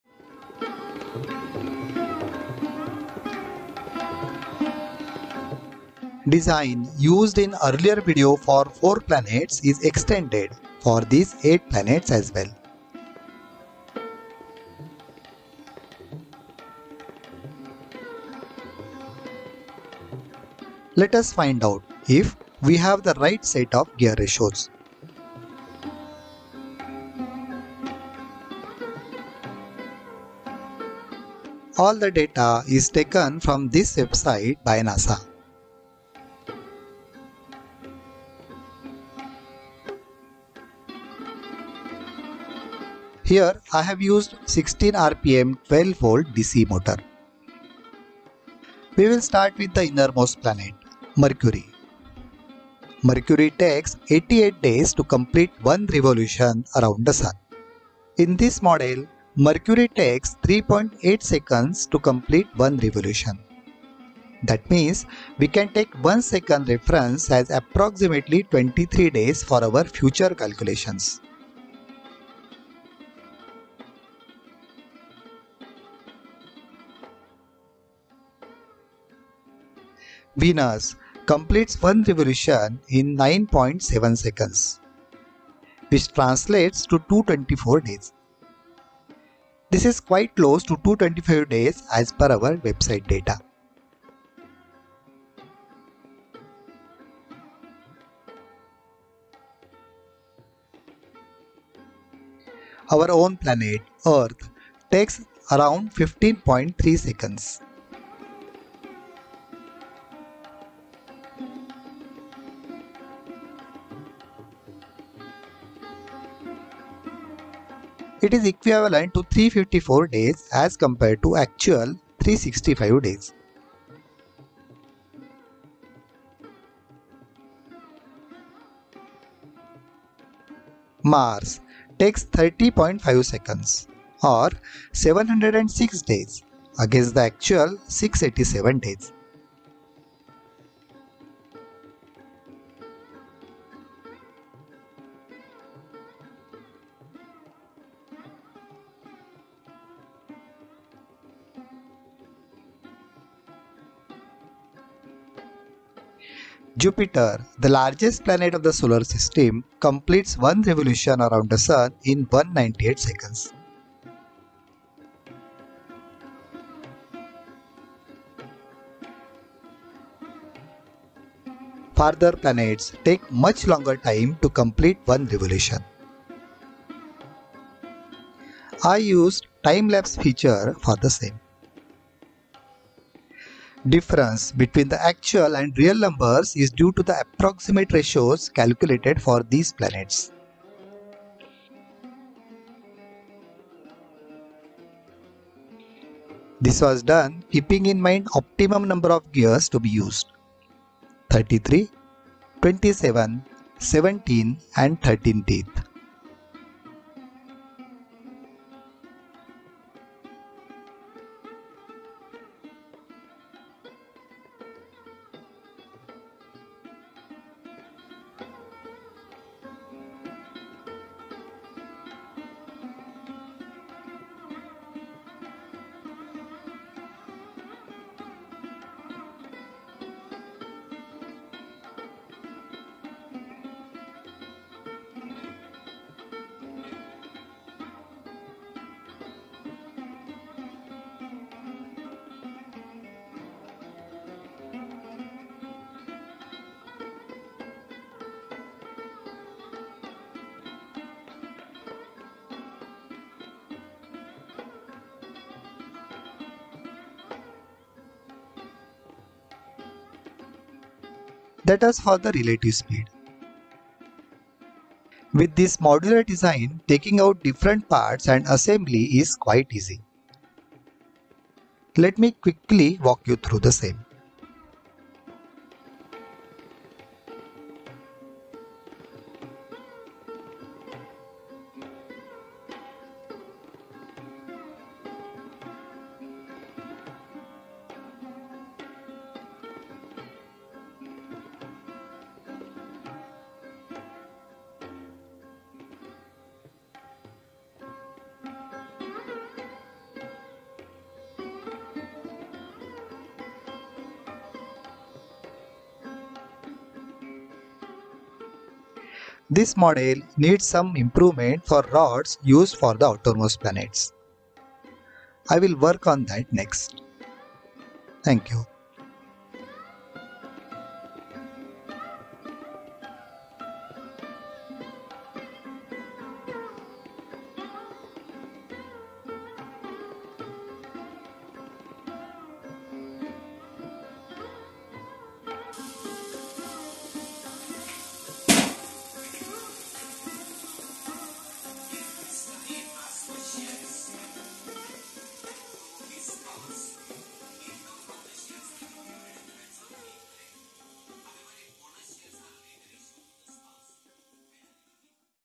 Eight Planets - Solar system - Working version
This new version has Eight planets of the solar system revolve around the SUN.

Design used in earlier video for 4 planets is extended to eight planets.

Let's find out if we have the gear ratios right.

All the data is taken from this website. By NASA.

We will start with the innermost planet Mercury which takes 88 days to complete one revolution around the SUN.

I have used a 16 RPM 12 V DC Motor.

In this model, it takes 3.8 seconds to complete one revolution. That means we can take 1 second reference as approximately 23 days for future calculations.

Venus, completes one revolution in 9.7 seconds in this model which translates to 224 days, quite close to 225 days as per NASA website.

Our own planet, Earth, takes around 15.3 seconds . It is equivalent to 354 days as compared to actual 365 days.

Mars takes 30.5 seconds OR 706 days against actual 687 days.

Jupiter, the largest planet of the solar system, completes one revolution around the SUN in 198 seconds.

Farther planets take much longer to complete one revolution. I used timelapse to cover the same.

Difference between actual and real numbers is due to approximate ratios calculated for the planets. This was done keeping in mind optimum number of gears to be used. 33, 27, 17 and 13.

That was for the relative speed.

With this modular design, taking out different parts and assembly is quite easy.

Let me quickly walk you through.

Model needs some improvement for rods used for outermost planets.  I will work on that next.